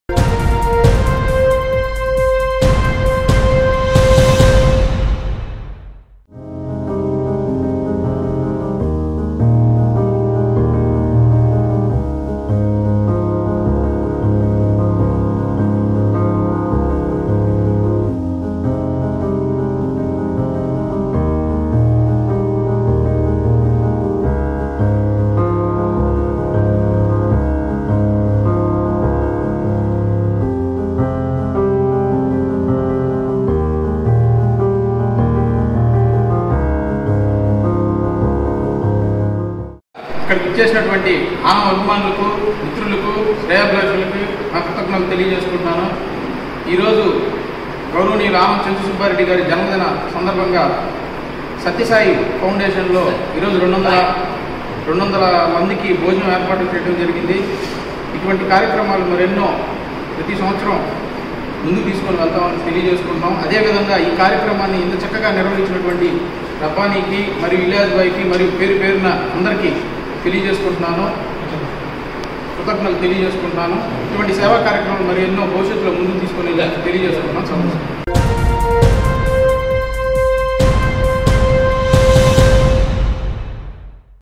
నెల్లూరులో ఏసీ బర్త్ డే.. సేవా కార్యక్రమాలు.. - NDN News
ఆనం చెంచు సుబ్బారెడ్డి జన్మదినం సందర్భంగా ఆయన అభిమానులు నెల్లూరులో సేవా కార్యక్రమాలు చేపట్టారు. సత్యసాయిబాబా ఆశ్రమంలో 200మందికి అన్నదానం చేసి.. మాస్కులు పంపిణీ చేశారు. అనంతరం కేక్ కట్ చేసి.. ఏసీ సుబ్బారెడ్డికి జన్మదిన శుభాకాంక్షలు తెలిపారు.